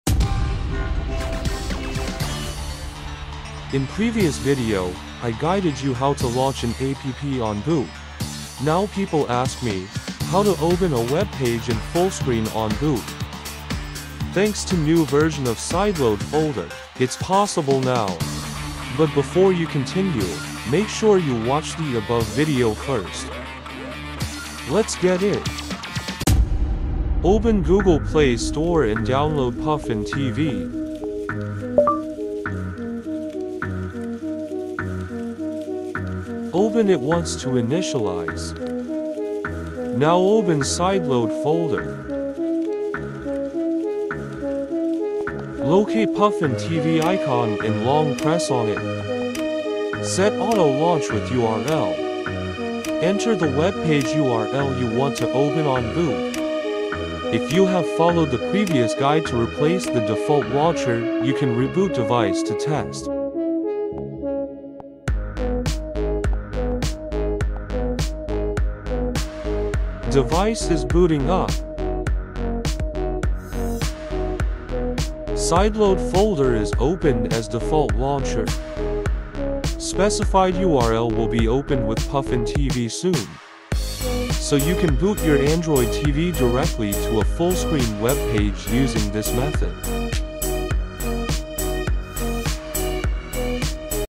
Open webpage URL in fullscreen on boot (system startup) in Android TV & Google TV
You can boot your Android TV or Google TV devices into a fullscreen webpage URL, turning your TV into a digital signage for any purpose as long as the content is presented or designed using web technology. Let's see how to do it with Sideload Folder.